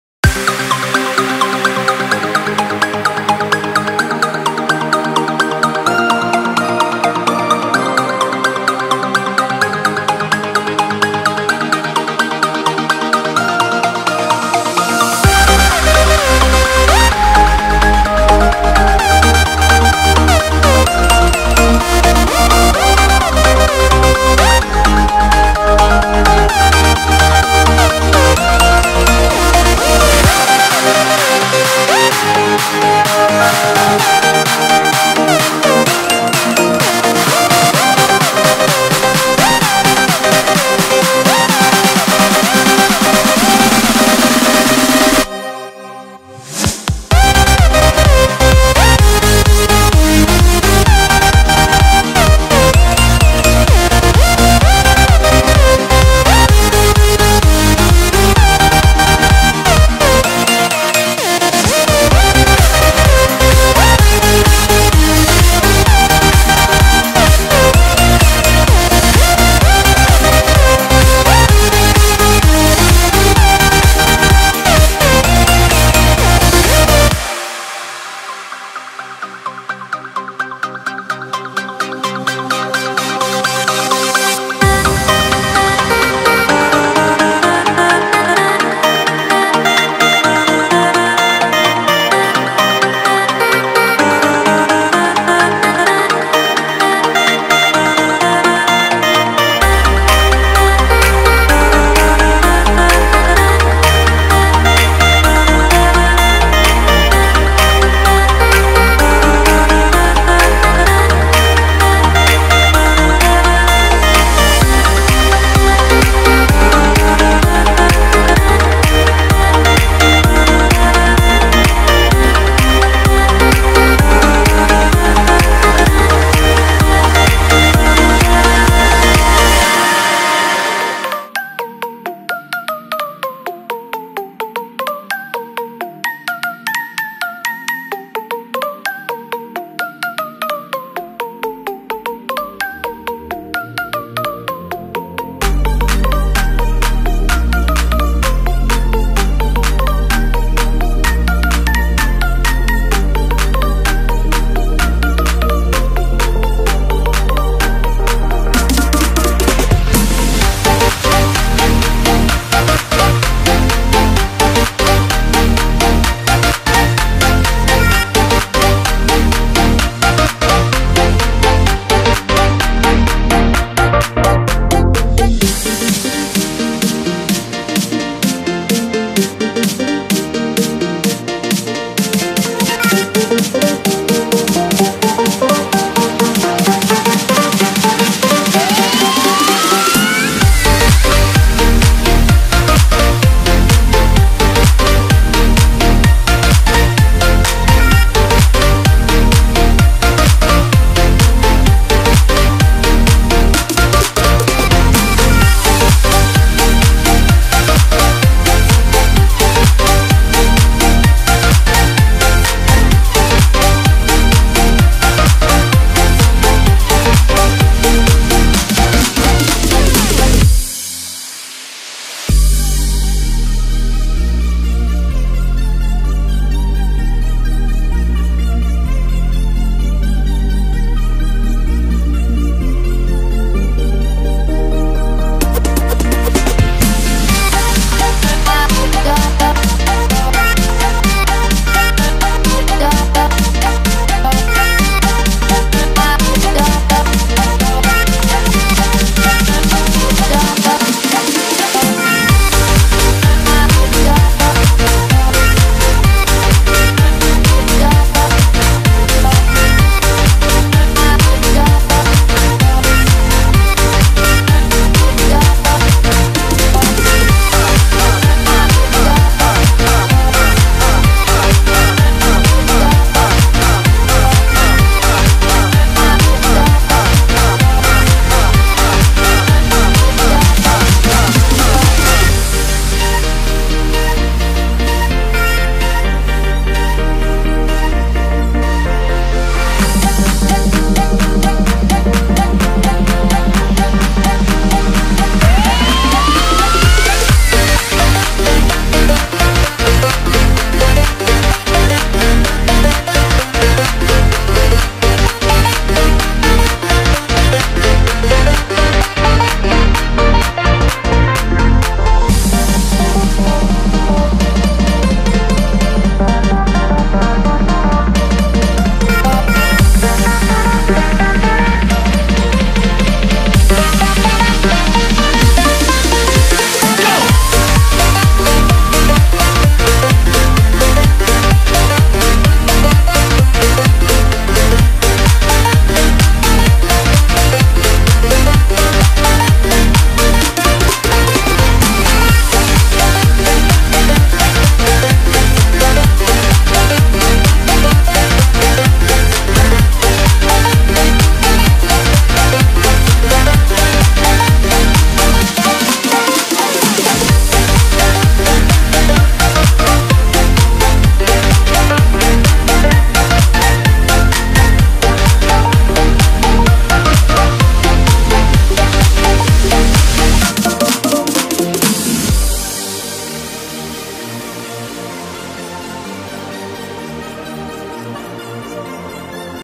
Siembra de cereal 2018//Sowing 2018
Resumen de la campaña de siembra de cereal 2018.
Claas Axion 810 Cmatic+ John Deere 750A.